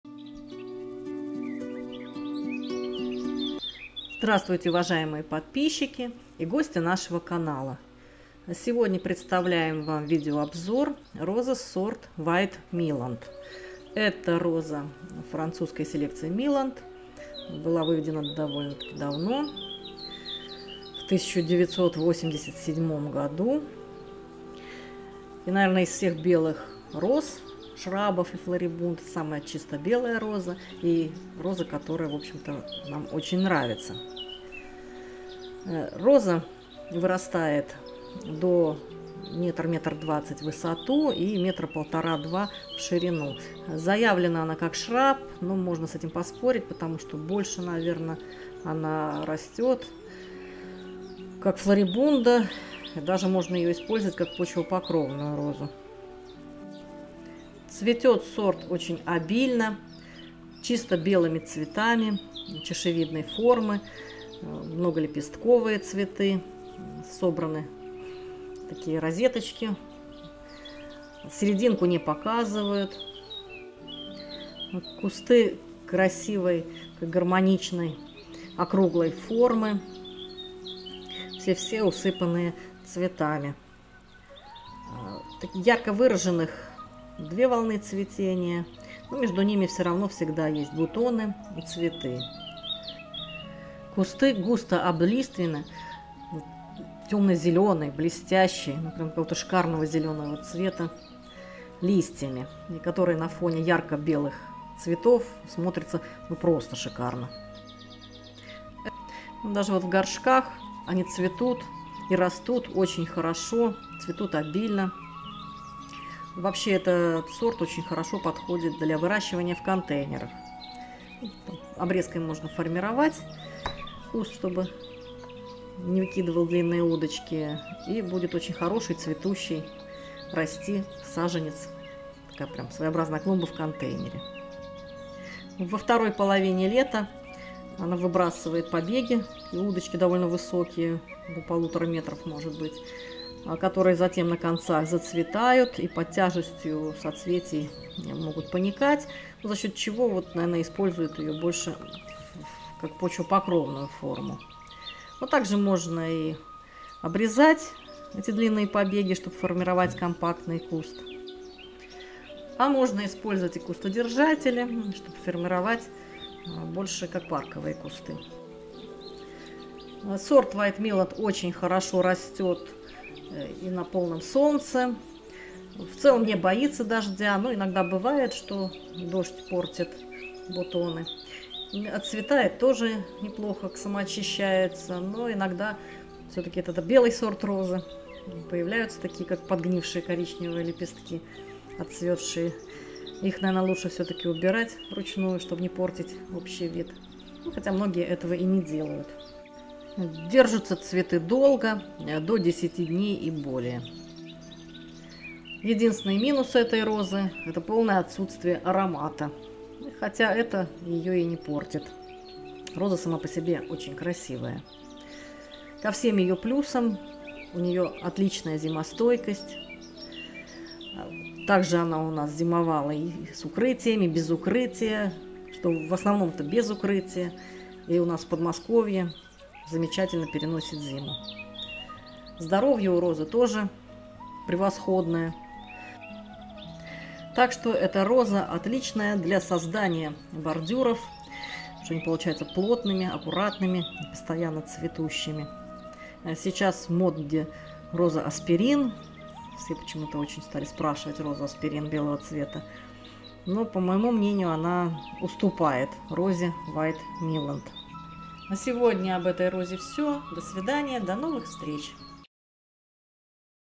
Видео обзор розы Бланк Мейдиланд - Blanc Meidiland (Meilland, 1987)
обзор розы   Бланк Мейдиланд White Meidiland (Blanc Meillandecor, MEIcoublan, Super Swany, White Meilland) Marie-Louise (Louisette) Meilland Франция, 1987

Цветки белые, махровые, среднего размера, 6-8 см, собранные в соцветия по 5-10 шт. Листья большие, тёмно-зелёные, блестящие. Куст раскидистый/ Побеги толстые, светло-зелёные, жёсткие, с многочисленными светлыми шипами. Полностью морозостойкий, обильно и продолжительно цветущий сорт. Отличное растение для посадки на газонах и в контейнерах. sadoved.com
Шраб
селекция: Meilland, 1987
цвет: белый
размер цветка: 7-9 см
высота куста: 80-150 см
ширина куста: 120-215 см
запах: *
устойчивость к мучнистой росе:
устойчивость к черной пятнистости:
устойчивость к дождю: ***